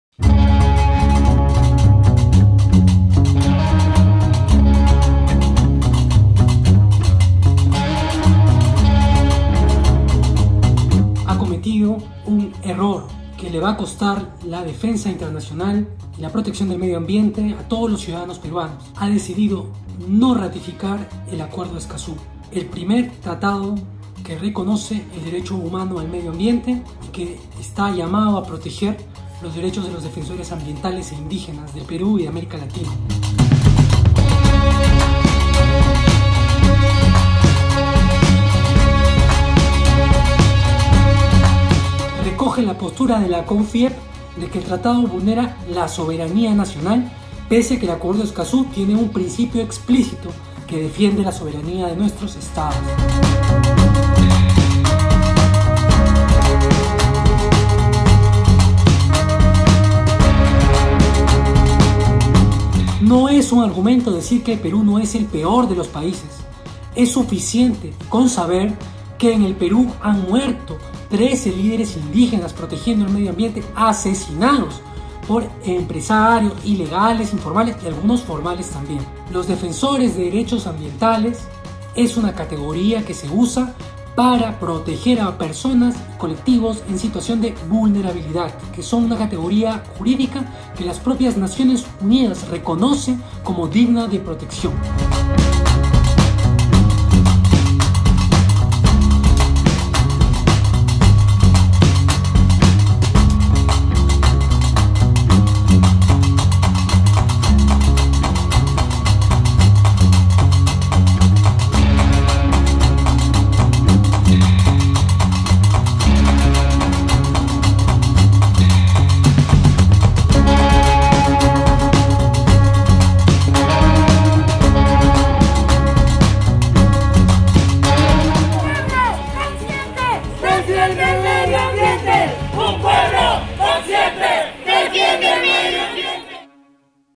Congreso rechazó Acuerdo de Escazú con mentiras
El Congreso repitió mentiras para rechazar el Acuerdo de Escazú y le dio la razón a las grandes empresas en contra del medioambiente.